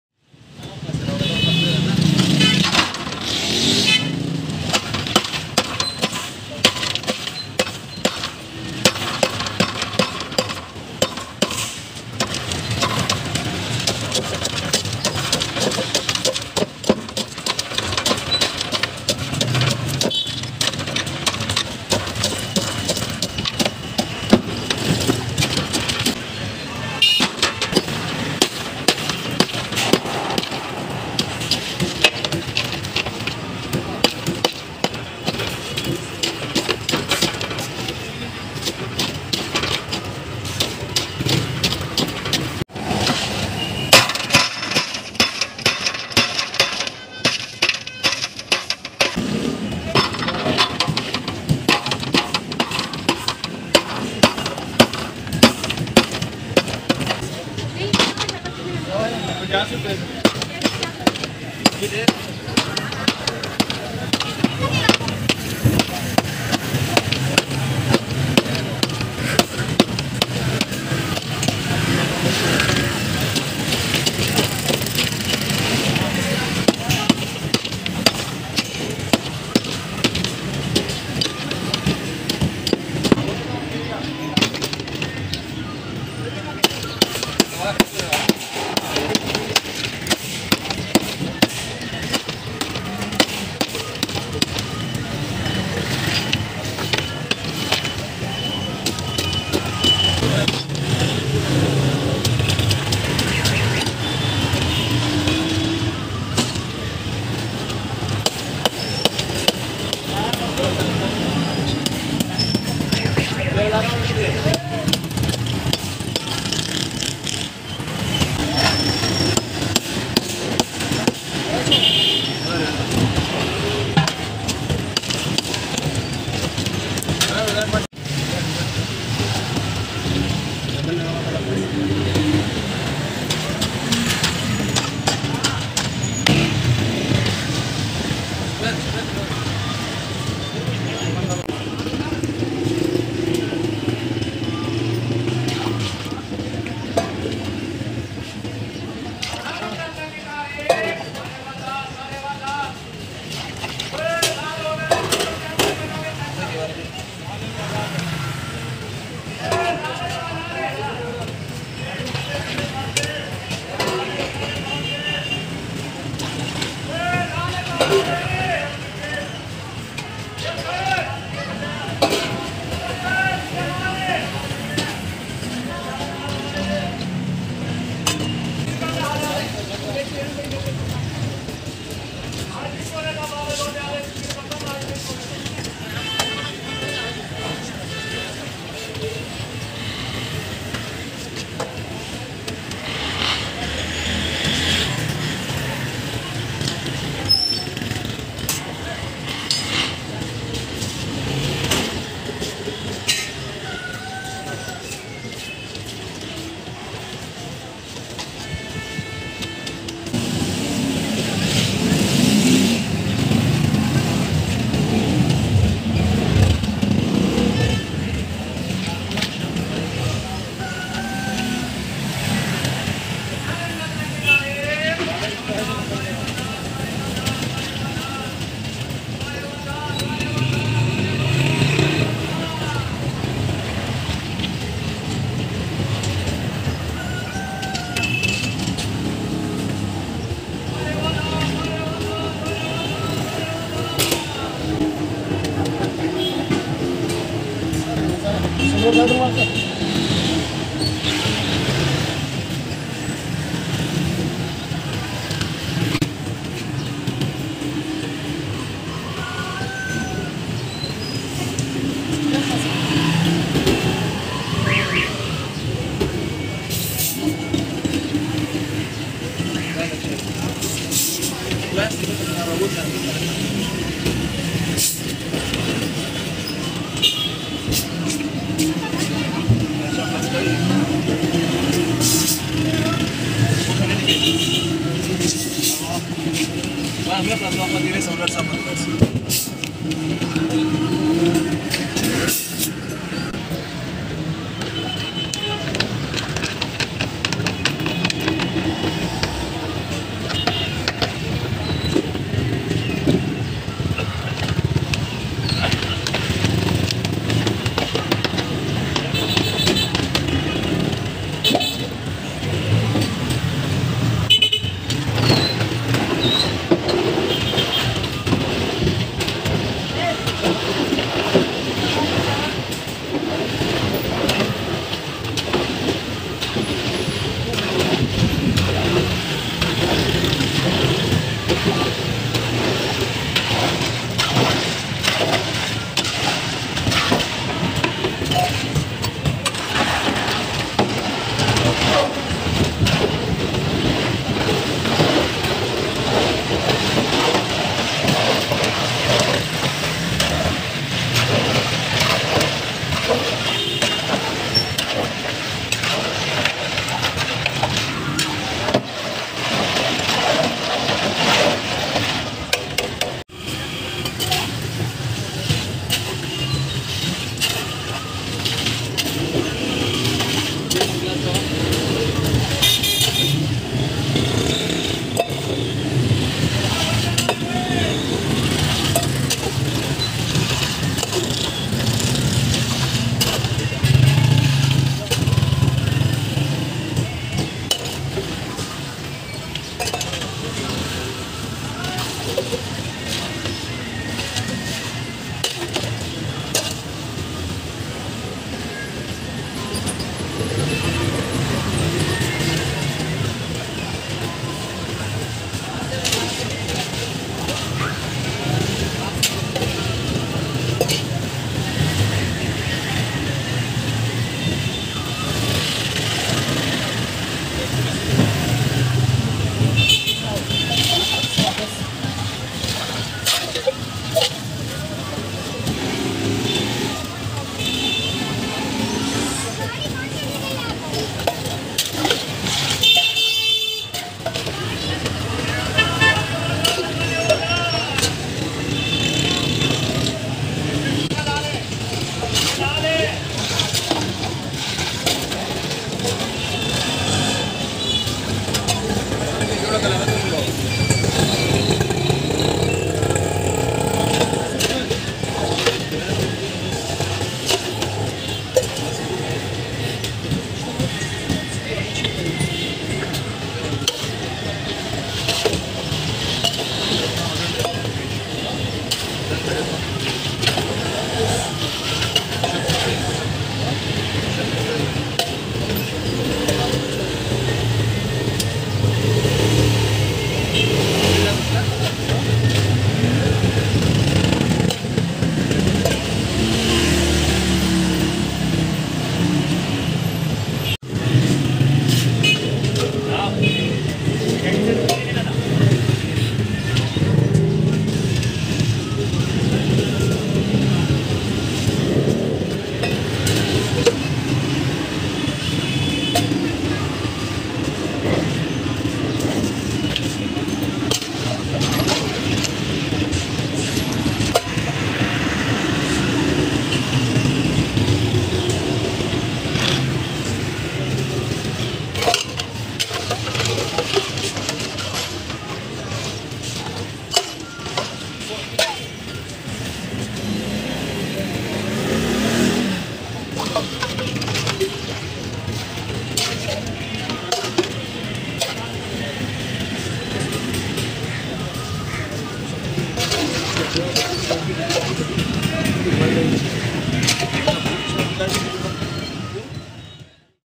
Pakola Milkshake | Refreshing Street Drink Pakola Milk Soda | Ice Pakola Juice. Karachi Street Food
Pakola Juice Making. Ice Pakola Milk Shake Summer Street Drink in Karachi. Pakola Doodh Soda Milkshake is Famous in Karachi Food Street. people love to enjoy Pakola Juice. Huge Making of Pakola Milkshake Karimabad Food Street of Karachi Pakistan

Address : Near United Bakery, Karimabad Block 3, Gulberg Town, Karachi, Pakistan.